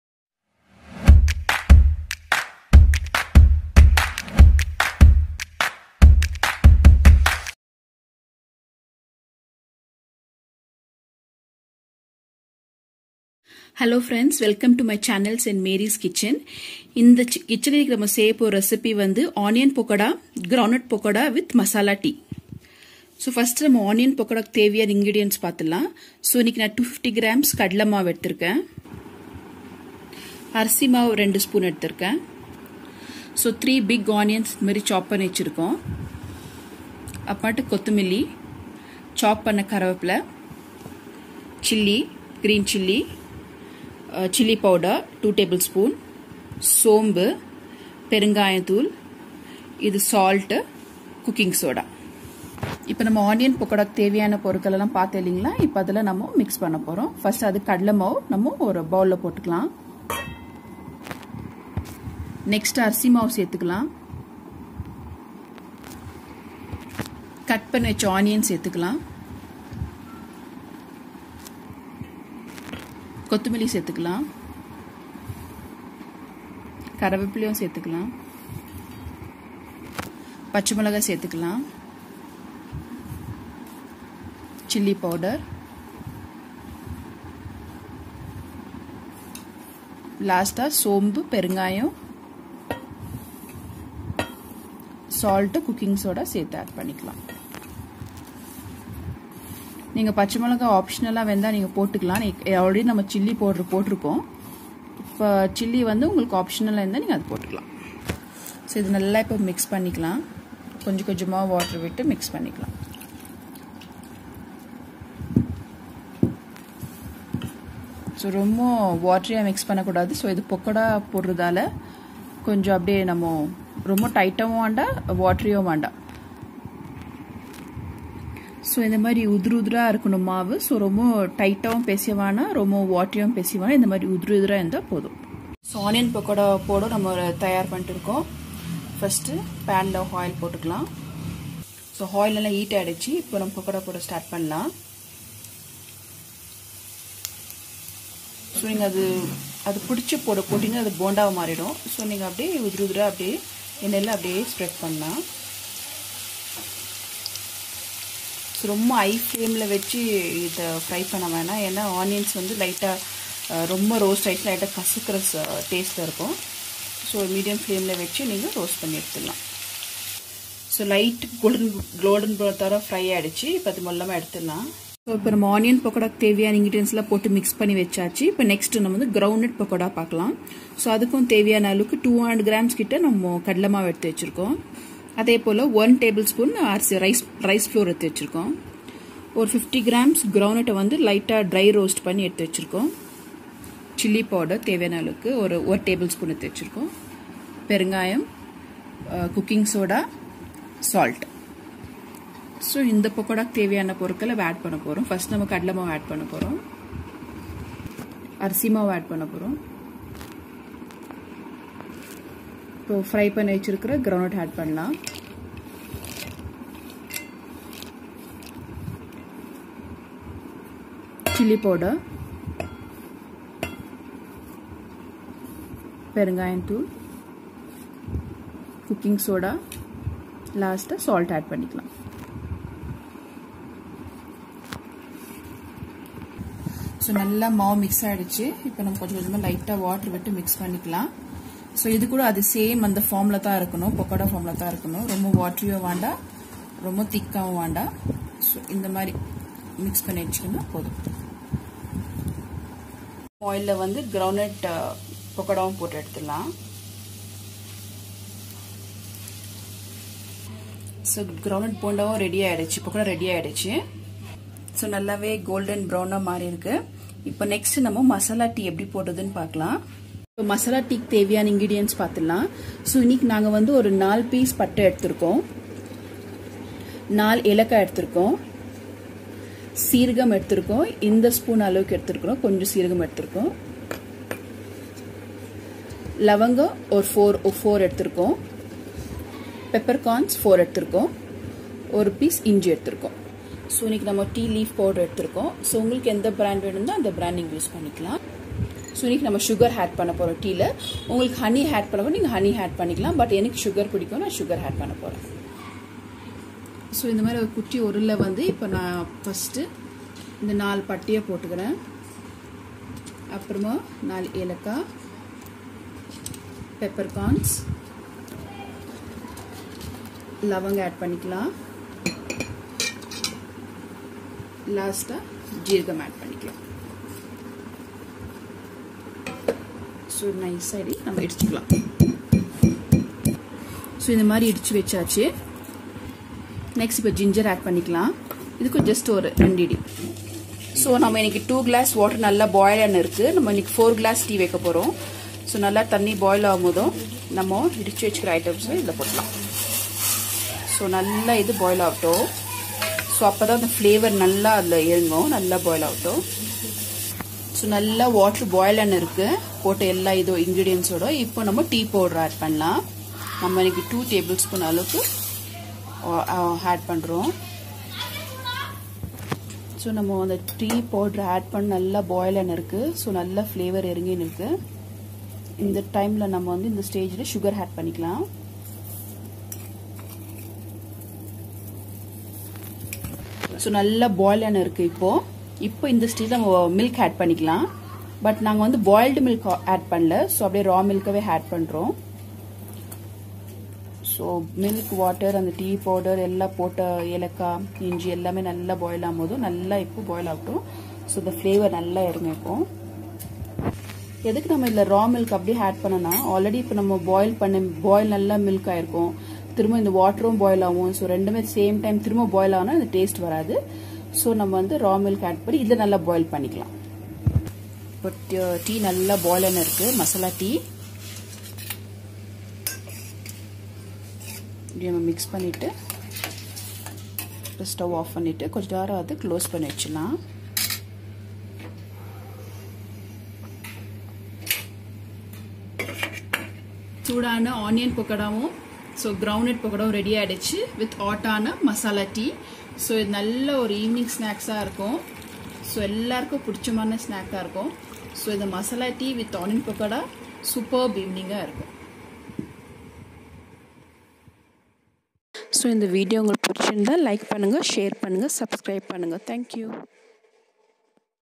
Crispy onion pakoda&Ground nut pakoda with hot Masala tea-Tamil
Best combo evening snacks ingerdients are :
Onion pakoda..
Big onion:2 No's
Green chilli: 5-6 No's
Curry leaves: 1 cup
Hing Asafoetida: pinch
Dry chiili powder :1 tsp
Coriander leaves : 1 cup
Fennel seeds: 1 tsp
Cooking soda: Pinch

Ground nut pakoda:
Roasted groundnut: 50 gms
Dry chilli powder: 1 tsp
Cooking soda: Pinch
Hing Asafoetida : pinch

Common ingredients  for two pakoda the dough is the same:
Besan powder
Rice flour.
And add above ingredients and can fry in the refined oil.

Masala tea:
Raw milk: 1 cup
Water : 2 cups
Leaf tea powder:2 tsp(Any brand)
Sugar: 3 tsp
Cinnamon stick: 4 small pieces
cardamon : 4 pieces
clove:4
Ginger: 1 piece
Jeeragum:  Little.

Enjoy your pleasant evening by adding some more flavours by Mary's kitchen.

We would appreciate your comments,shares and likes.